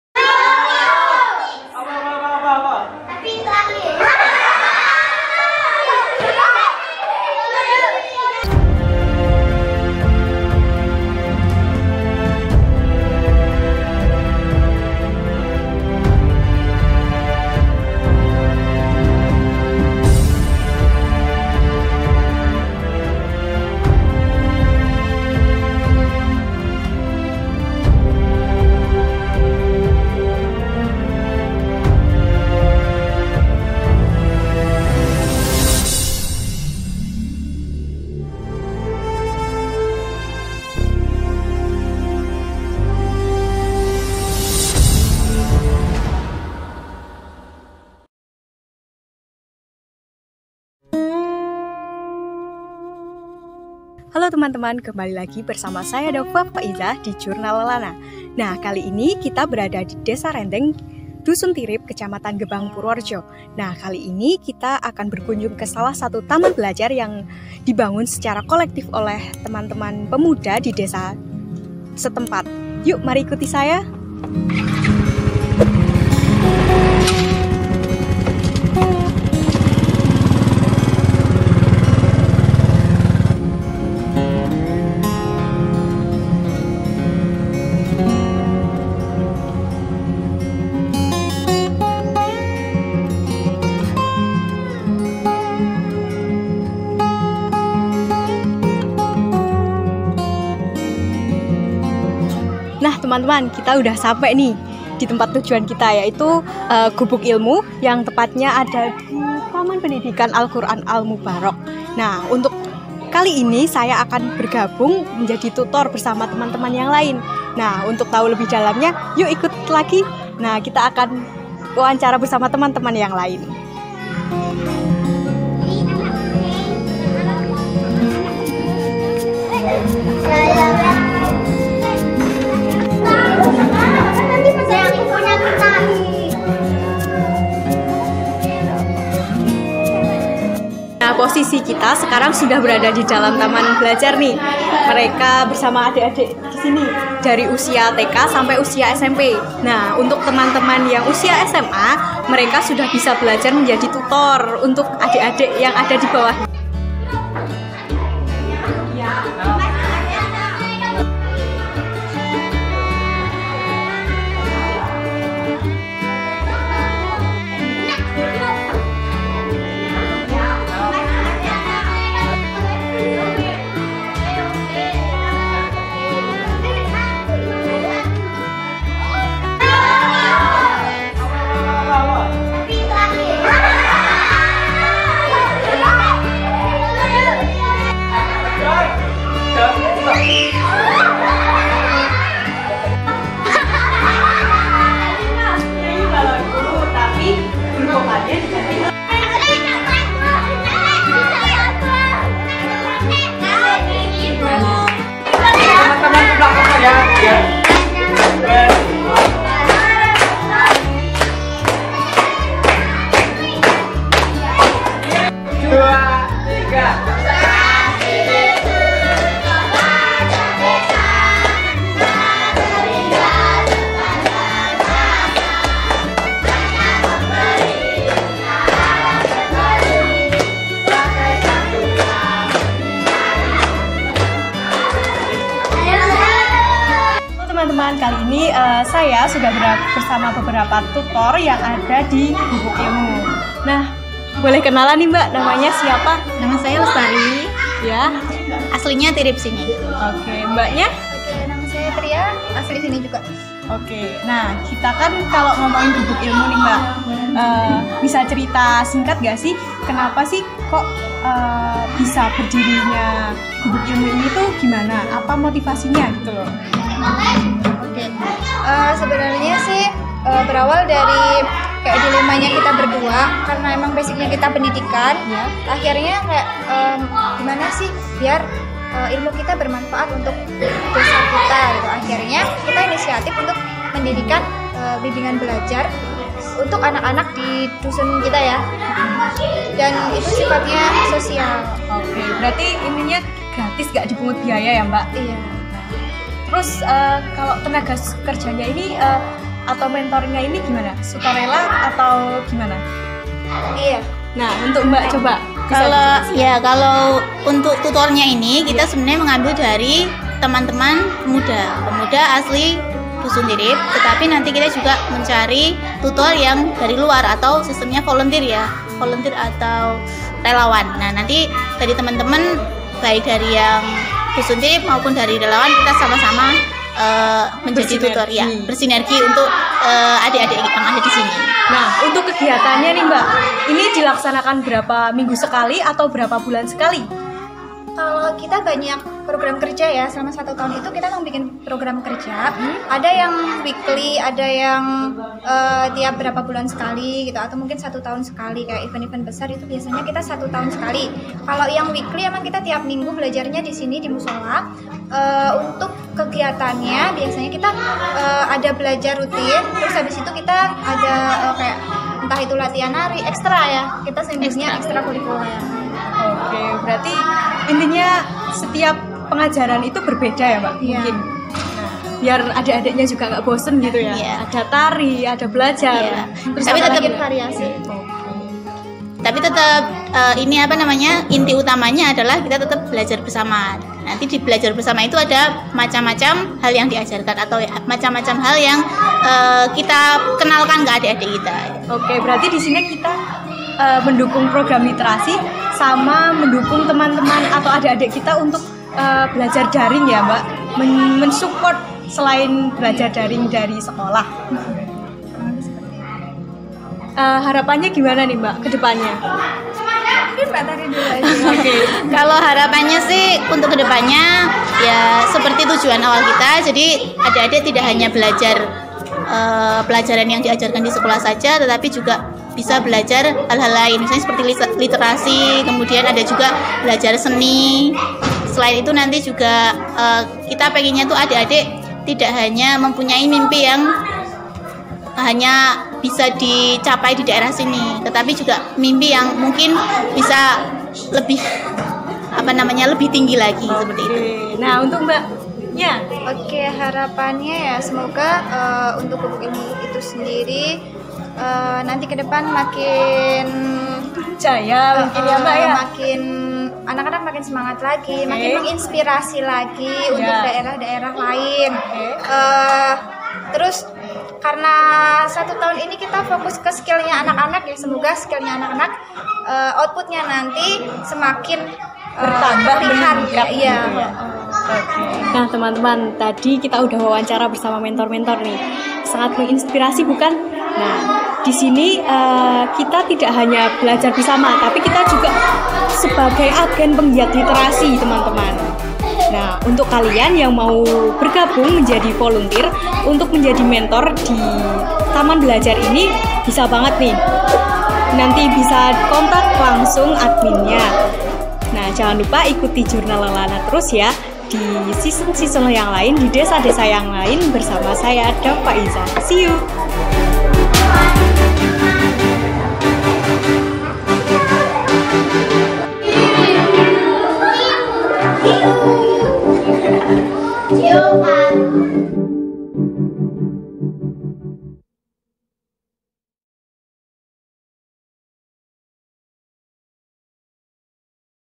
Gubuk Ilmu, Lentera Literasi di Tengah Desa Rendeng
Gubuk ilmu berada di Dusun Tirip, Desa Rendeng, Kec. Gebang, Kab. Purworejo. Menfasilitasi anak-anak Masyarakat setempat untuk berkegiatan belajar mengajar.

Gubuk ilmu menjadi wadah anak-anak dalam mengembangkan minat bakat dan juga kreativitas dengan memanfaatkan kekayaan alam lingkungan setempat.